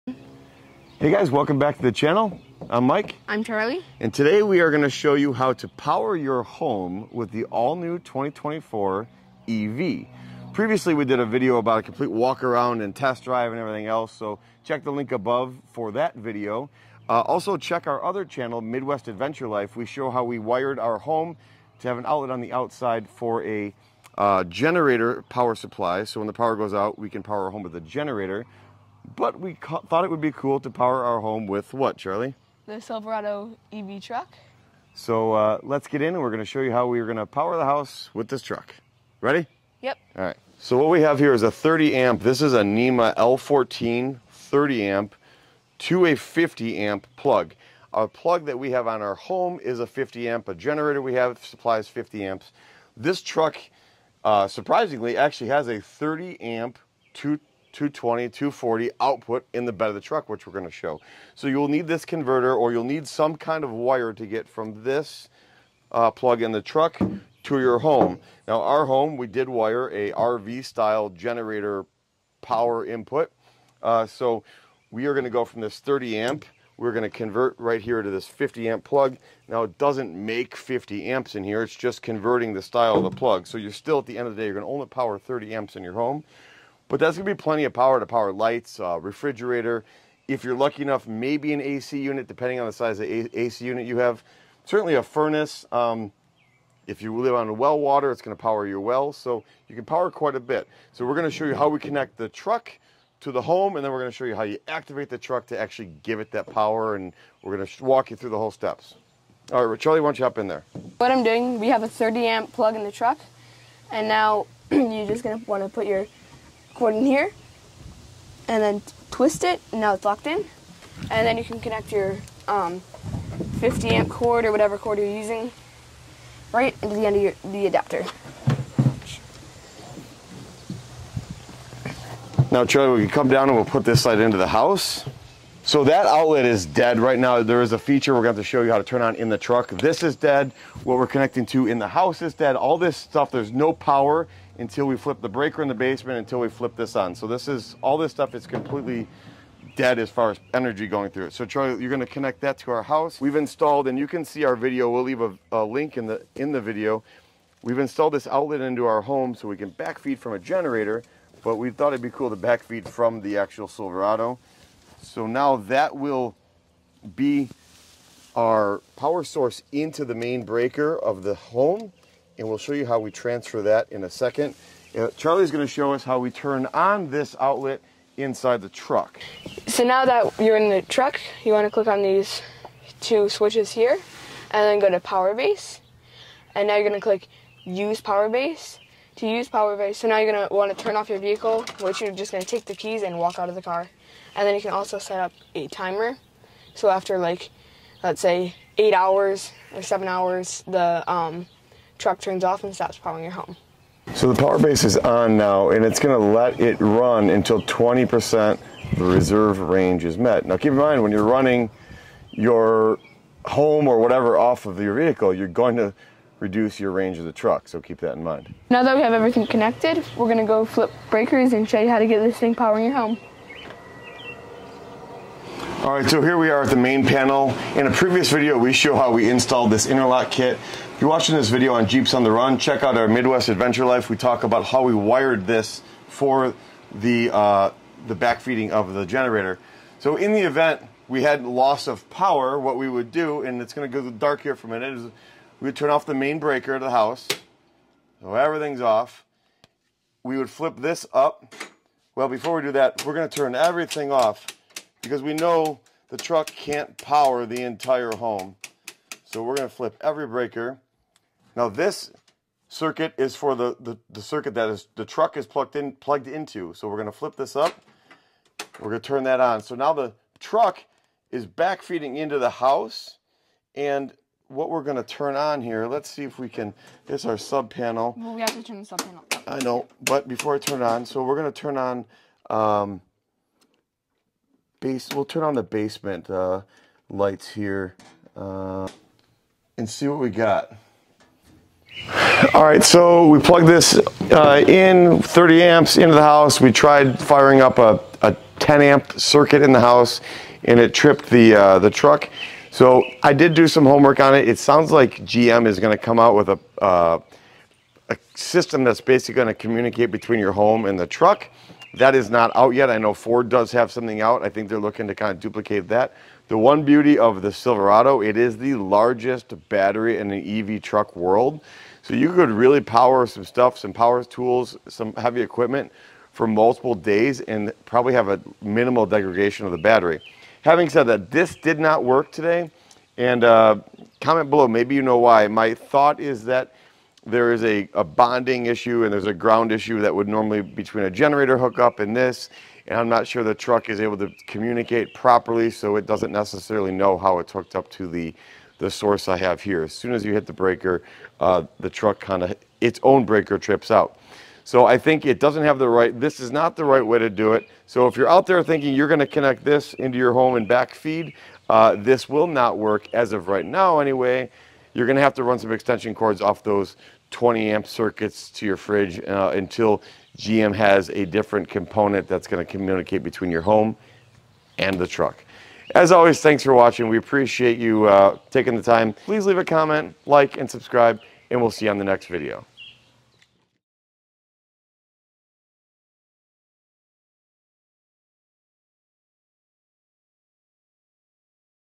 Can You Power Your Home With An EV Silverado? | Chevy PowerBase | 30 Amp 240v Service
Today we try powering our home with the all new Silverado EV.  Have you see the new PowerBase system?